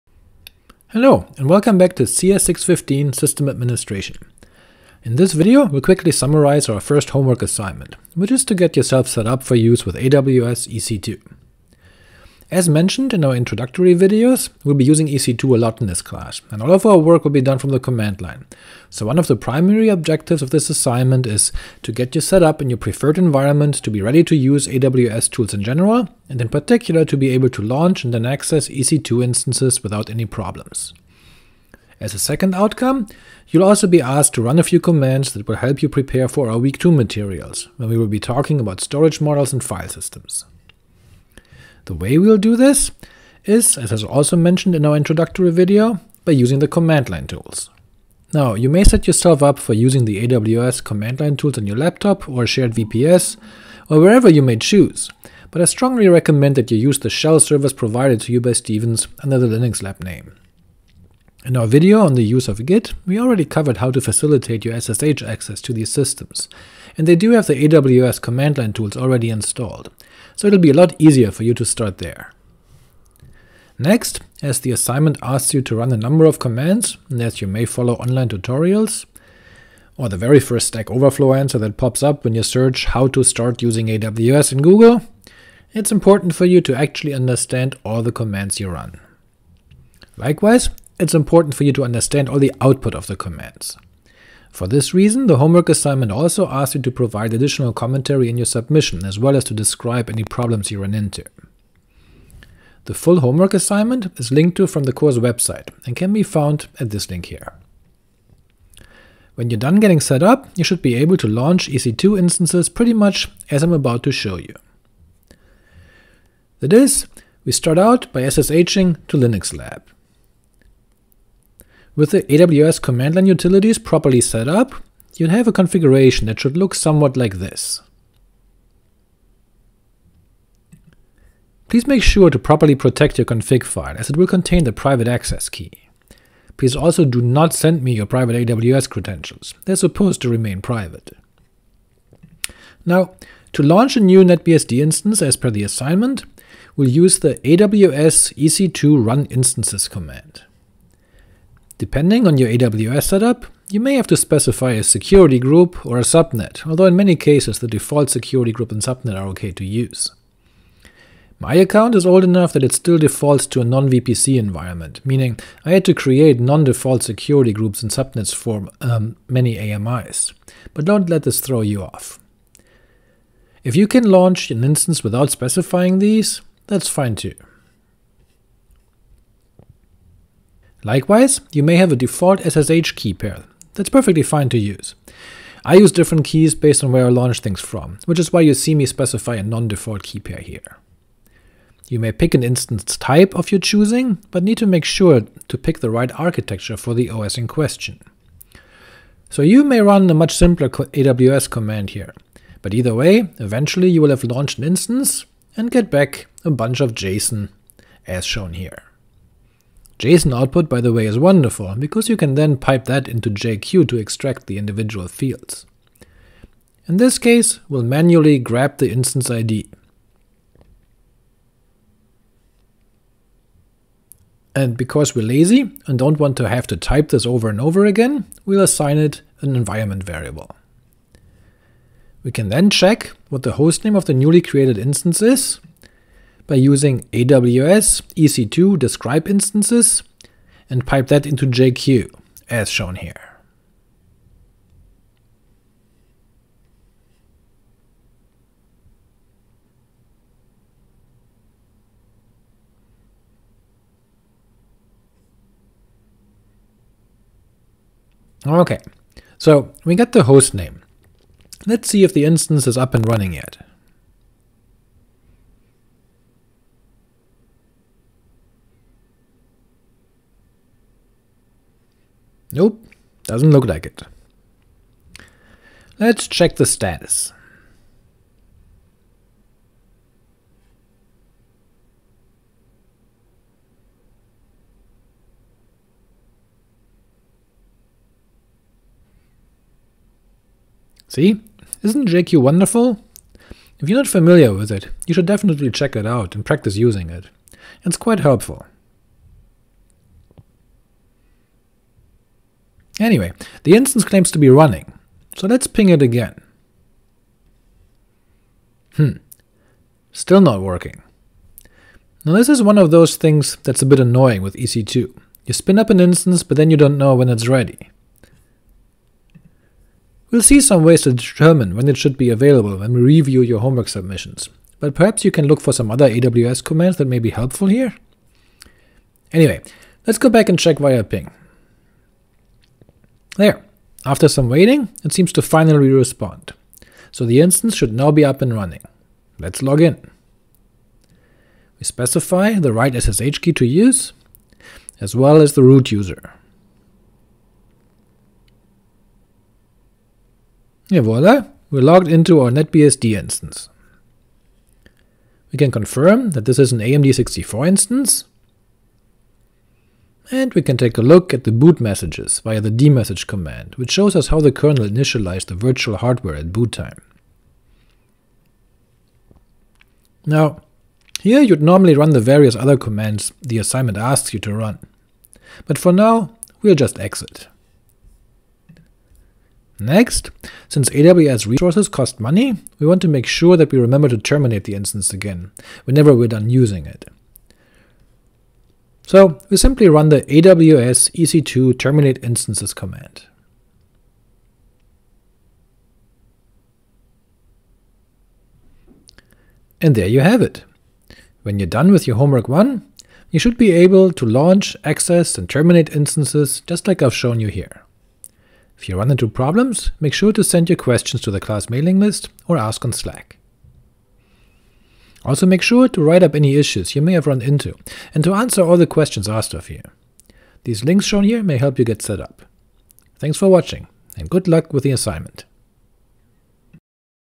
CS615 System Administration, Week 01, Segment 06 - Homework 1: Warming up to EC2
In this video, we'll quickly summarize our first homework assignment, which is to get yourself set up for use with AWS EC2.

Outline:
00:00 Introduction
01:55 Sample AWS invocations
06:54 Summary